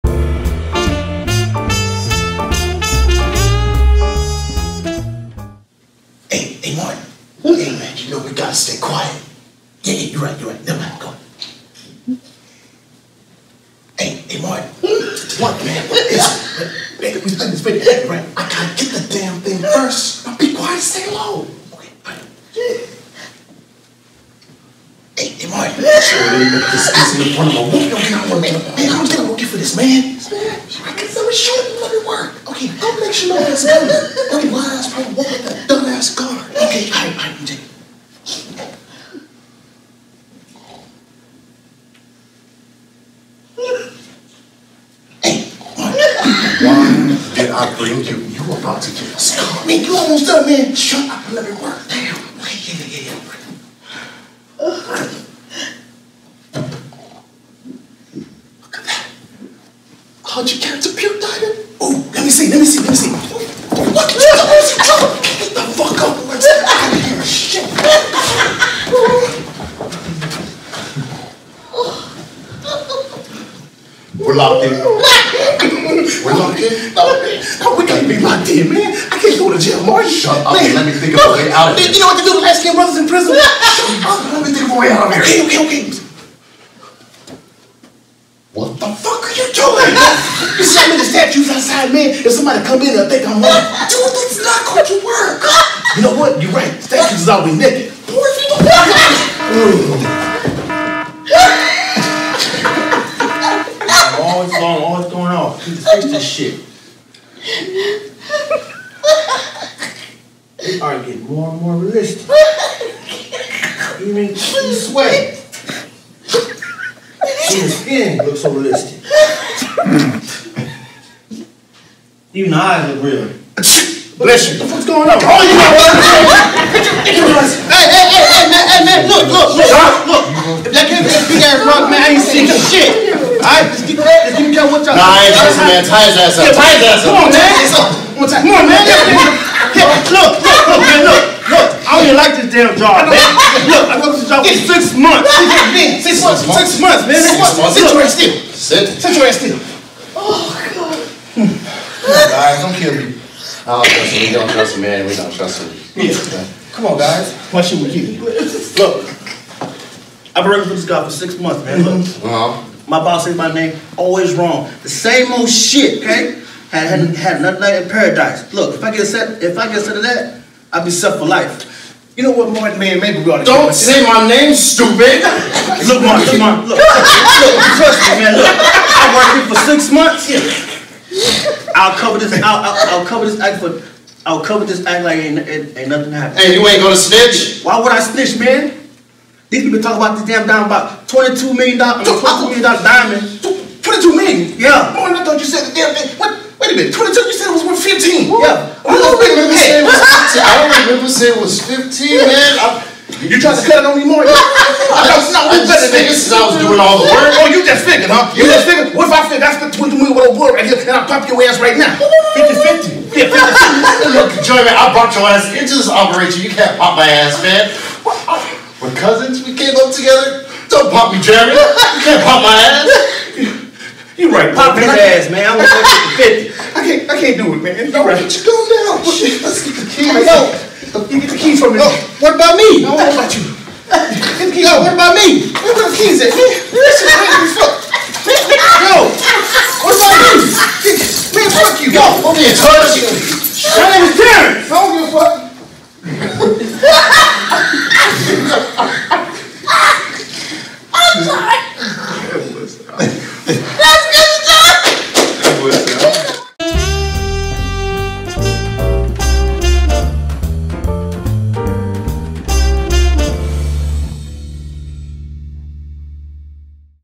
Jewel Thieves
Bumbling thieves Jeremy and Martin attempt to steal a diamond.
Charnele Brown Acting Presentation
Houston, Texas
1-9-2021